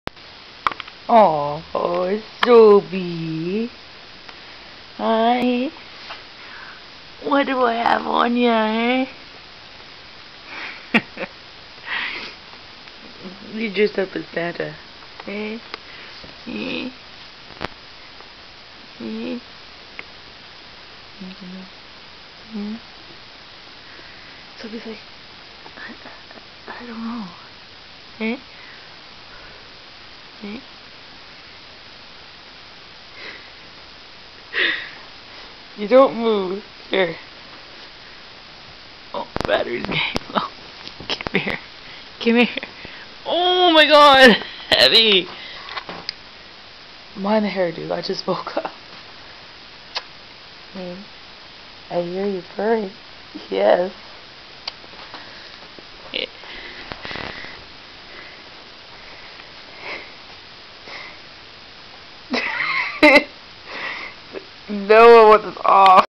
My handsome little man Soubi dressed up as santa. but man he needs to loose some weight lol